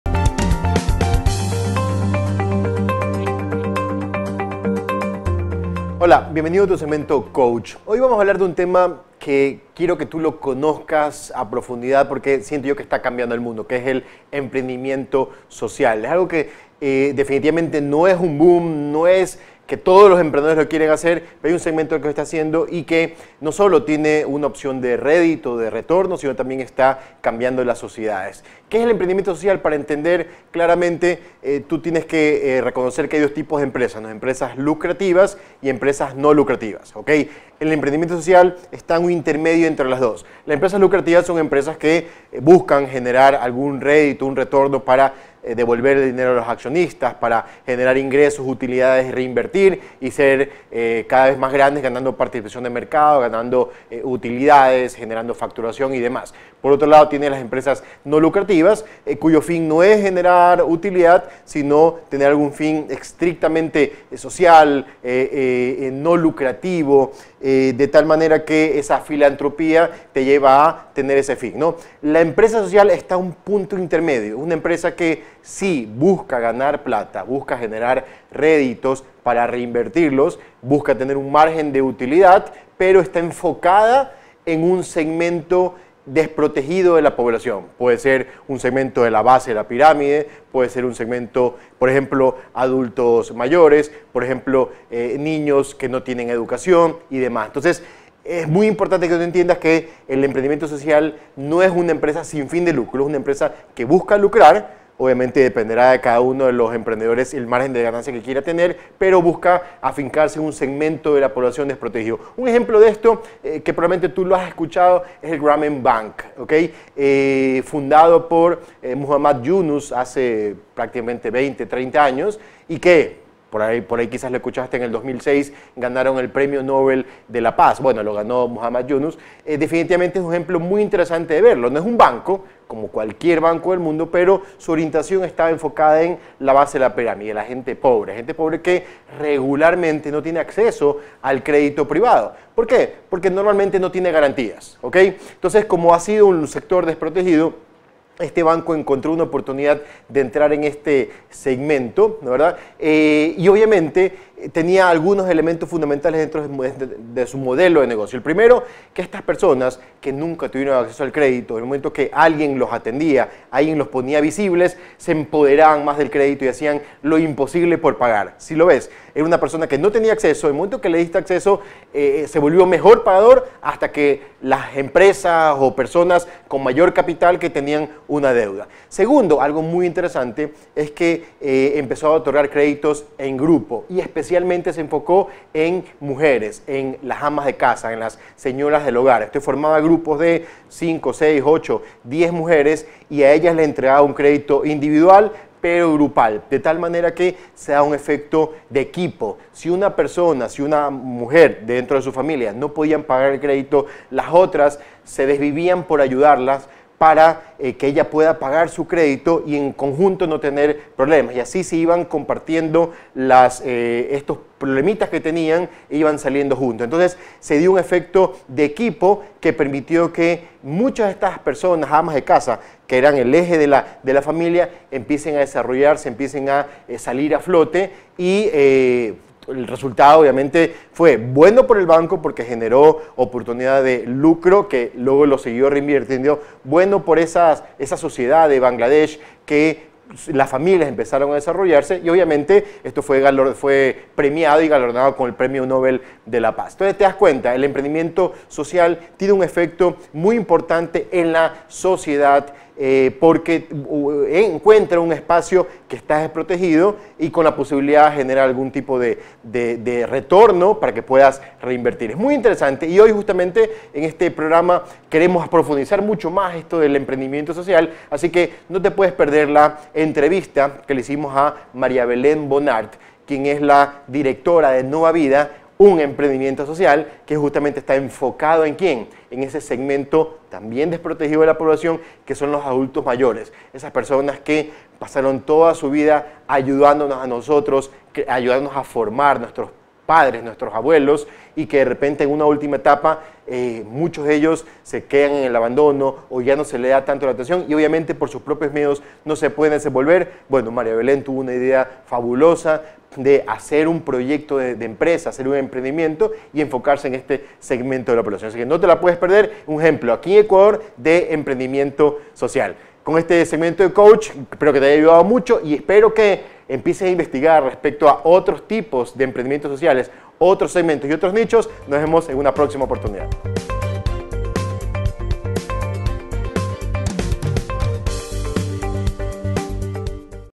¿Qué es el emprendimiento social?
¿Sabías qué el emprendimiento social está cambiando el mundo de los negocios?
Te enseñaremos que es, para que sirve y como lo puedes poner en práctica para que comiences a emprender en esta área.